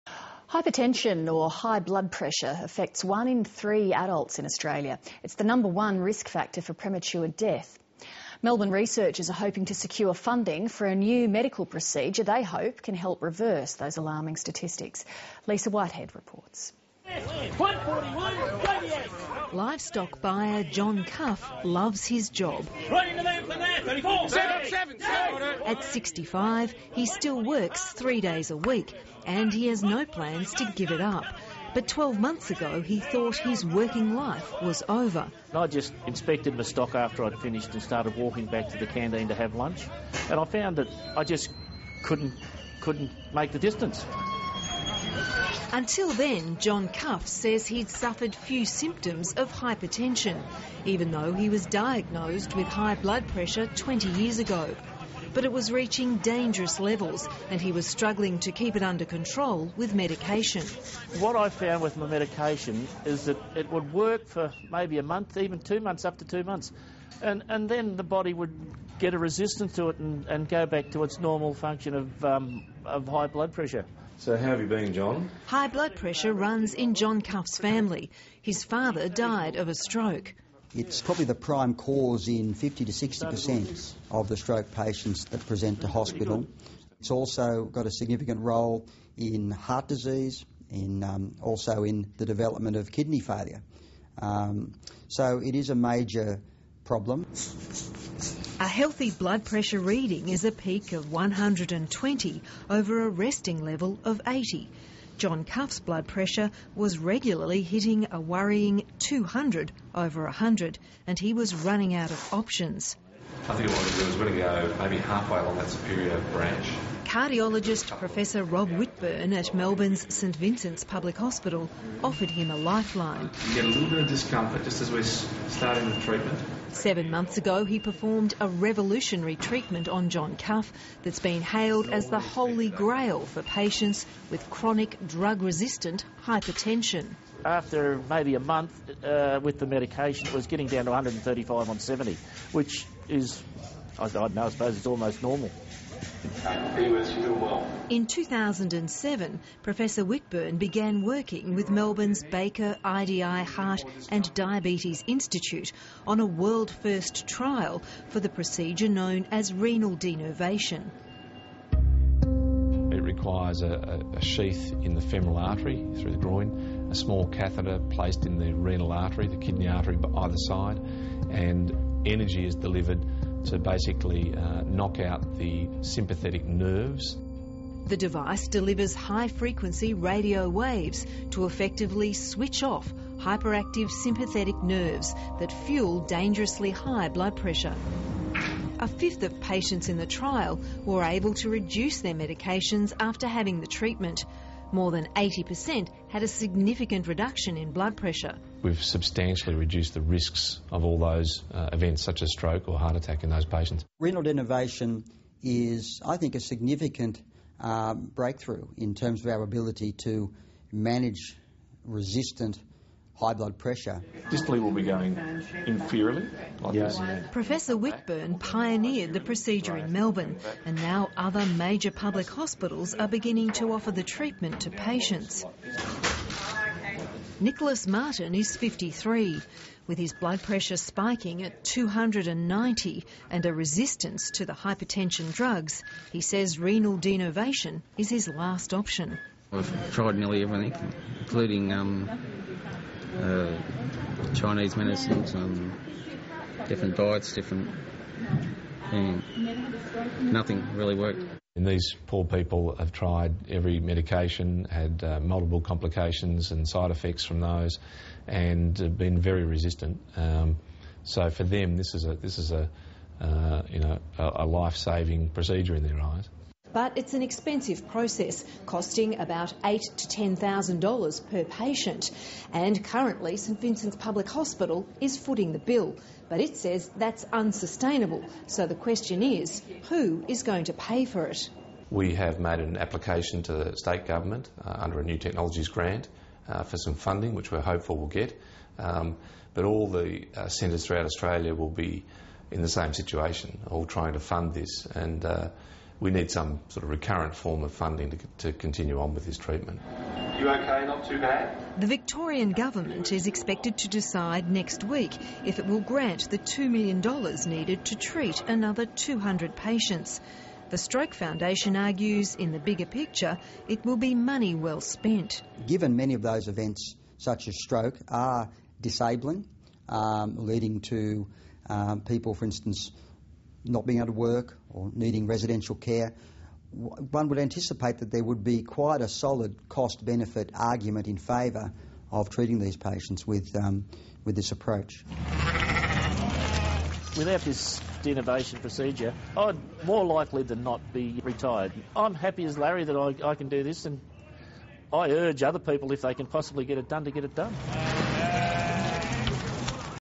New treatment for hypertension
Melbourne researchers are striving to secure funding for a revolutionary therapy for hypertension - the number one risk factor for premature death in Australia. Lisa Whitehead investigates